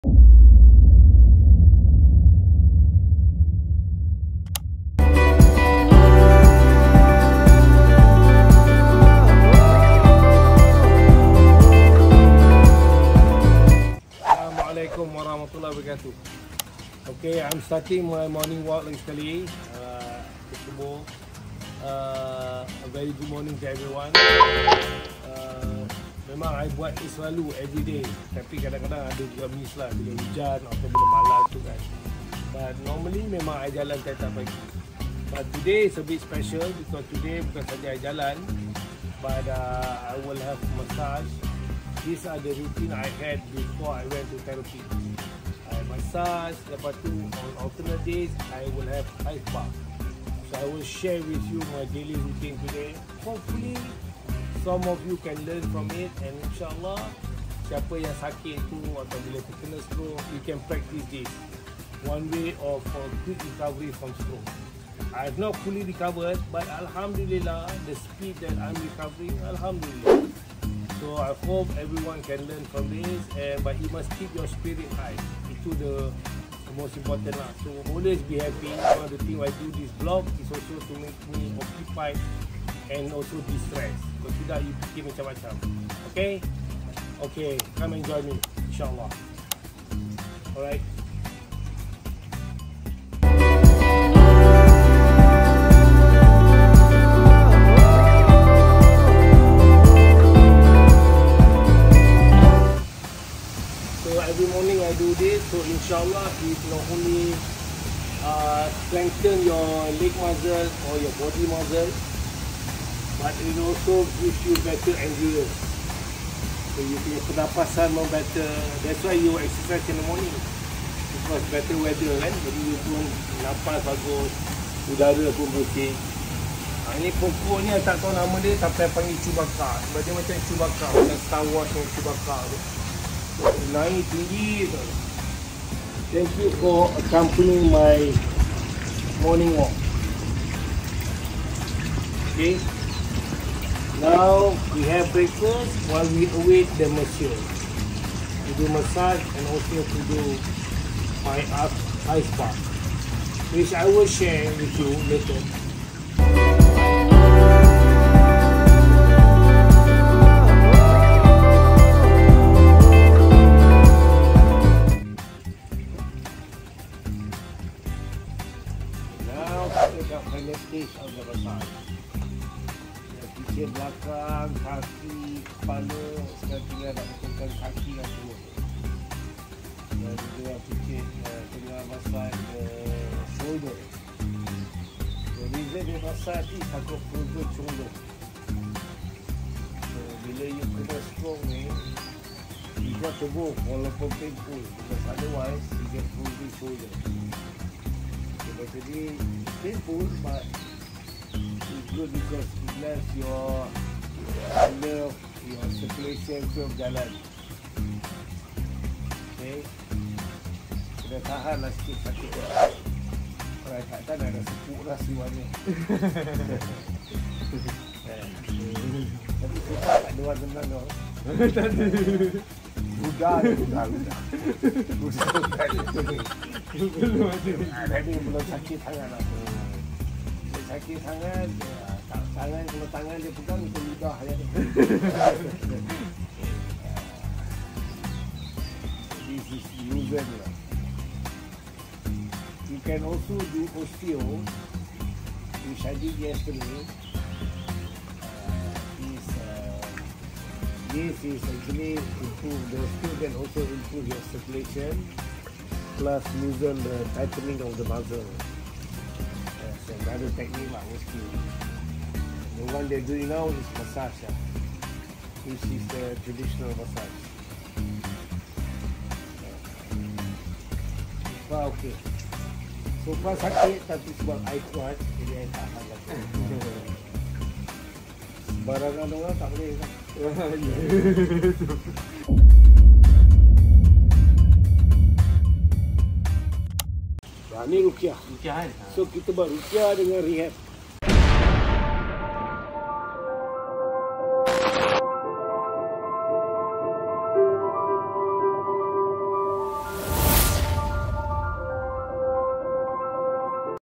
Road to Recovery from Stroke Part I
Sharing my morning routine of rehabilitation in the hope to fully recover from stroke in syaa Allah 🤲 We must remain strong and discipline to build back our muscle mass or it gets harder to recover